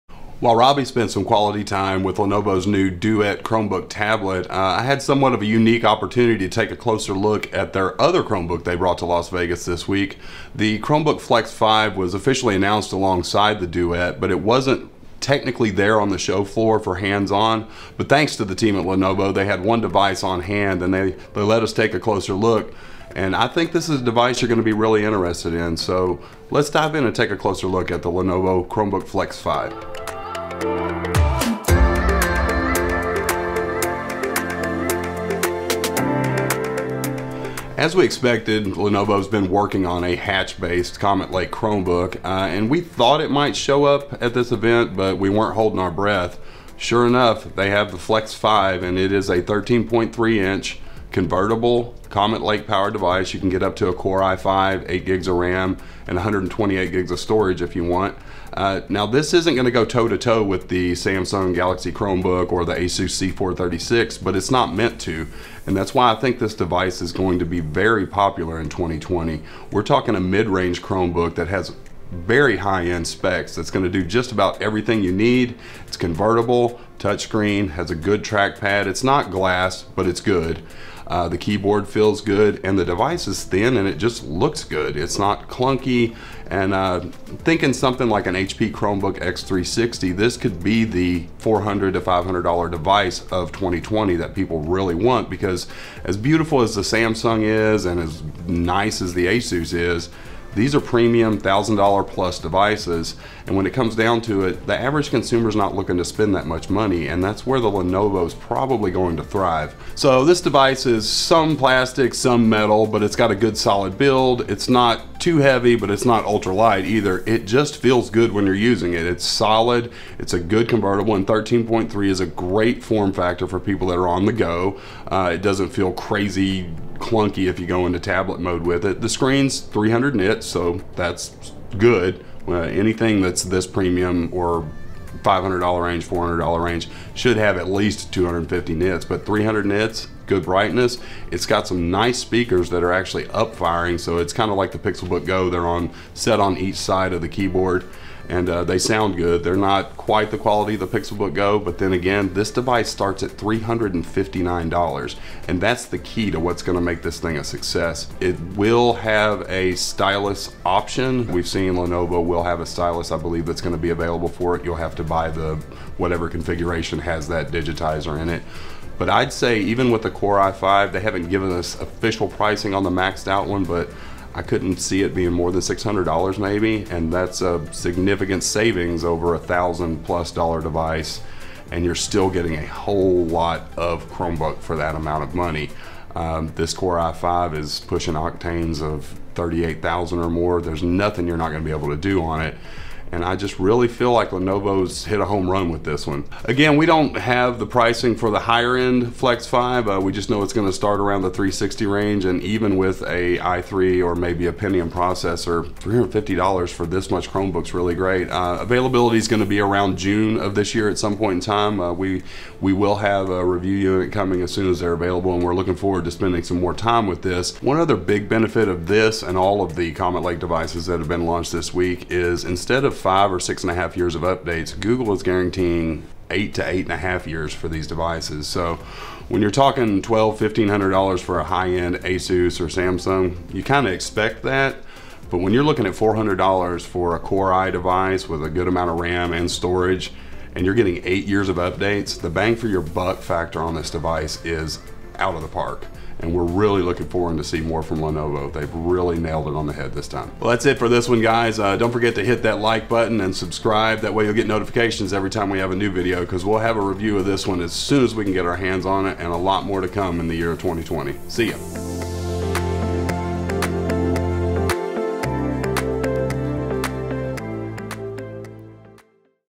Lenovo Chromebook Flex 5: Hands-on and First Impressions
The Lenovo Chromebook Flex 5 didn't come to Vegas to compete with the Samsung Galaxy Chromebook or the ASUS Flip C436 and that's okay. As we've seen over the past couple of years, the average consumer is much more interested in a solid device with some flagship-caliber features that keep the price tag around $500 or less and that's exactly why the Flex 5 is poised to be a massive success. With a starting price of only $359, the Flex 5 Chromebook should experience the same type of consumer popularity that we saw with the HP Chromebook X360 14 that offered a premium experience with powerful specs and could frequently be found on sale for less than $450.